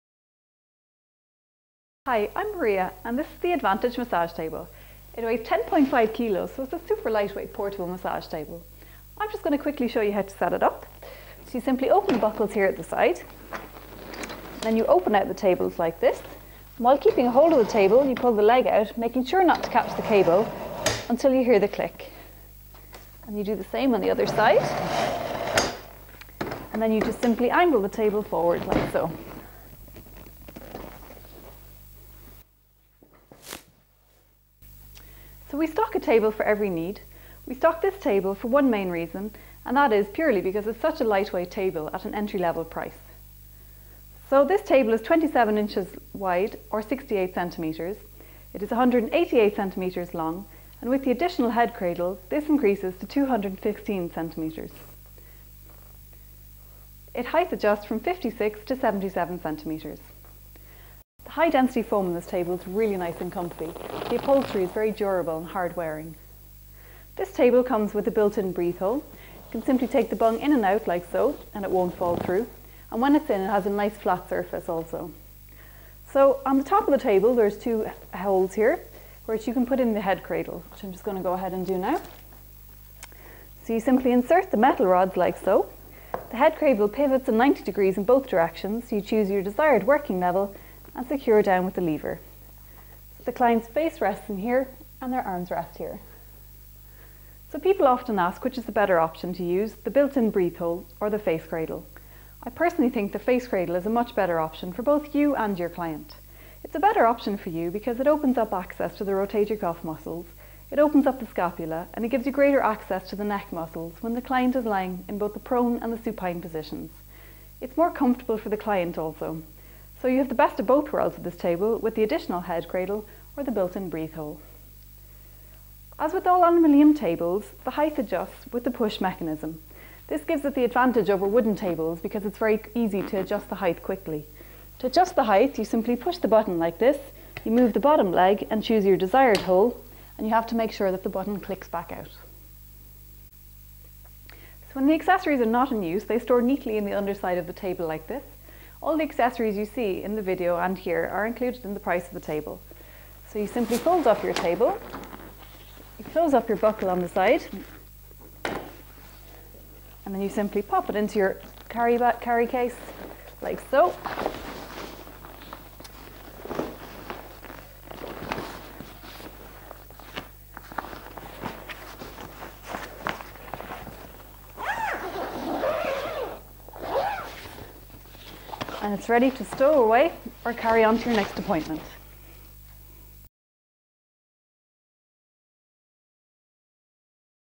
Review of The Advantage I MASSAGE TABLE from Massage Warehouse UK
The Advantage I aluminium massage table weighs only 10.5kg (23 lbs). Other comparable tables on the market will cut down on the width of the table to reduce the weight. The Advantage table is a full 27in (68.5cm) wide and 188cm (74in) long. Do not sacrifice any client comfort by reducing the width of the table! All clients will feel comfortable and secure on this mobile massage bed.

Receive with purchase of this massage couch a face cradle, head cushion, arm sling, built in breathe hole and durable black carry case. You are able to choose between using the face cradle or the convenient built in breathe hole when treating patients. The height of the Advantage bed can be adjusted in just seconds with the push button height adjustment.

This table will suit a variety of treatments and is a great value table for those starting out their career or any therapist looking for a portable massage table.

We offer a 30 day no questions asked return policy and a 1 year warranty.